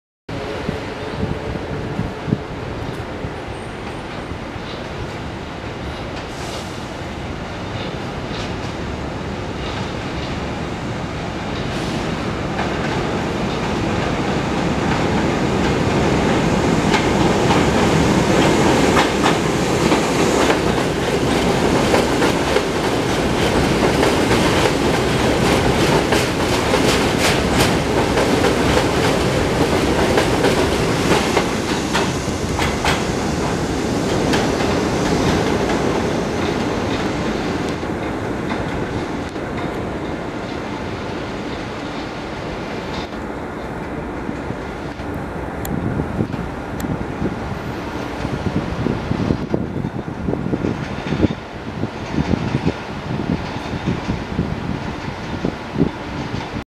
R62A going to Coney Island Yard by the West End D line
bypassing 62 street

This was taken on 8/19/2019